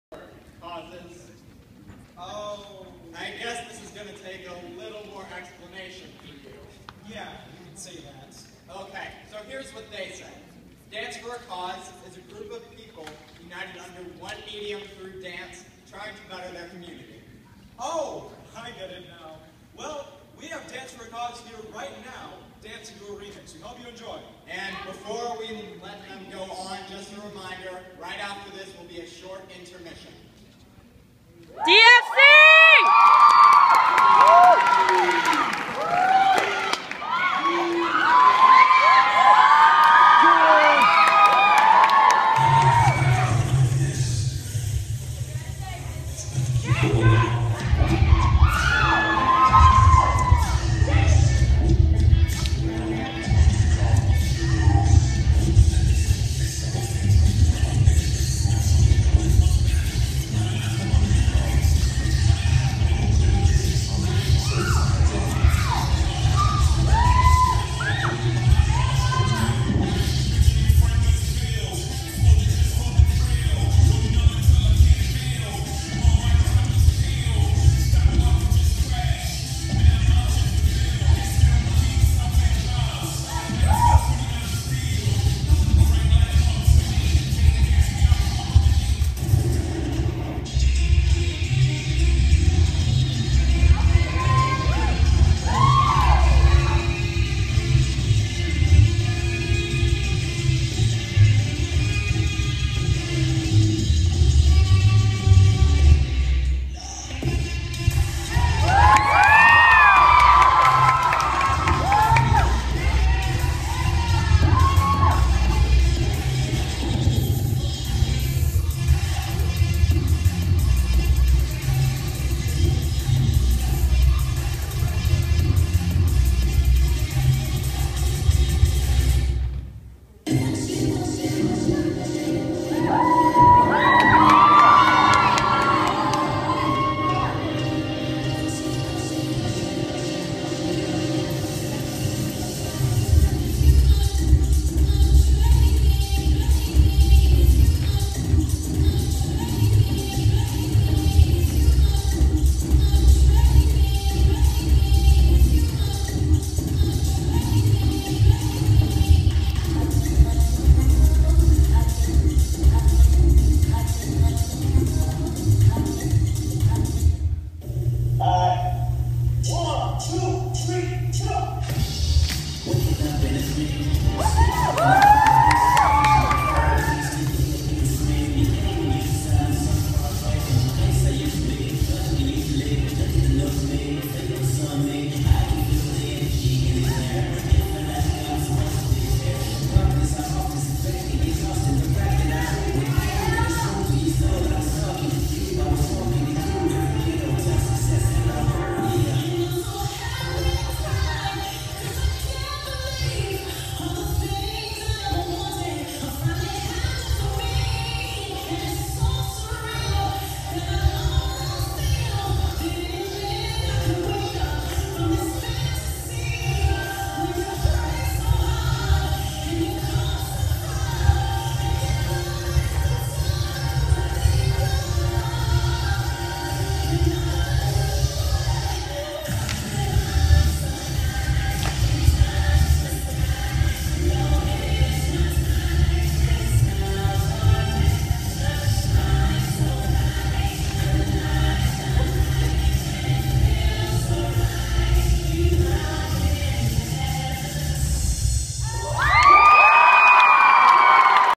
Jasper Talent Show '14: DFC
Dancing For a Cause
Jasper High School Talent Show 2014
1/22/14